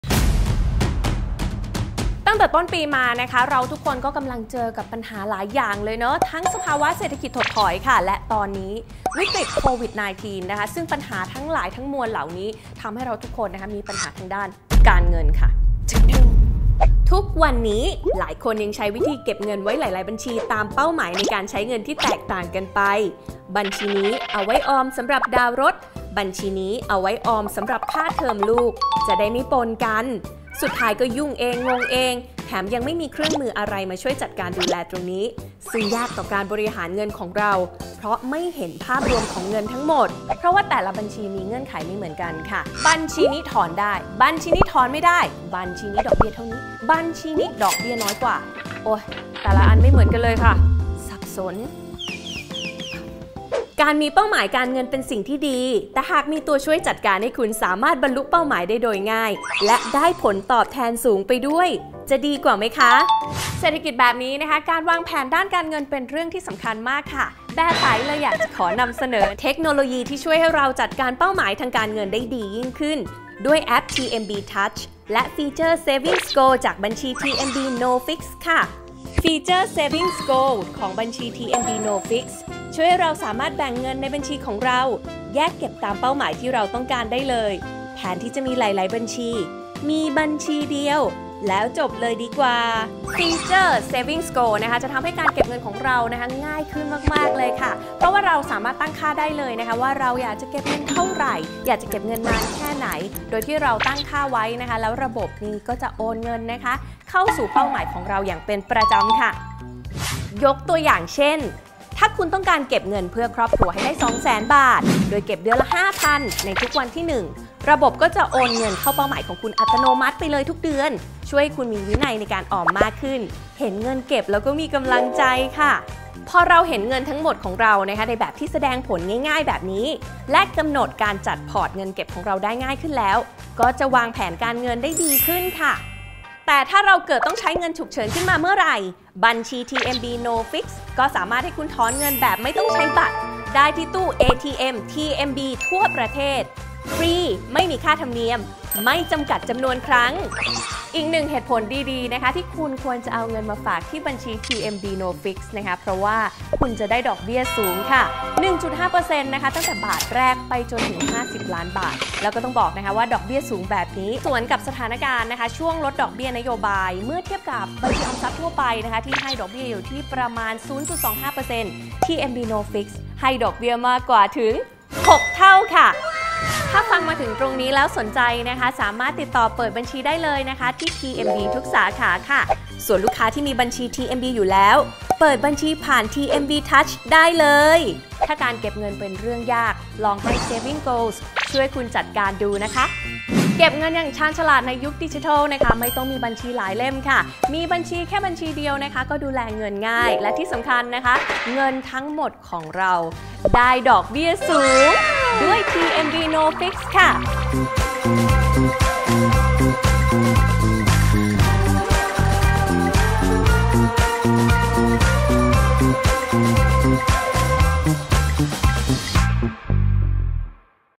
TMB No Fixed ตัวช่วยให้รอดพ้นวิกฤติการเงิน!
วิกฤตหลายอย่างที่เข้ามาในตอนนี้ ช่างทำให้การเก็บเงินของเราขาดความคล่องตัวยิ่งนัก และด้วยเหตุนี้ แบไต๋เลยอยากที่จะนำเสนอตัวช่วยดี ๆ อย่าง TMB No Fixed ที่จะทำให้การออมเงินเป็นเรื่องง่ายและคุ้มค่าในตอนนี้!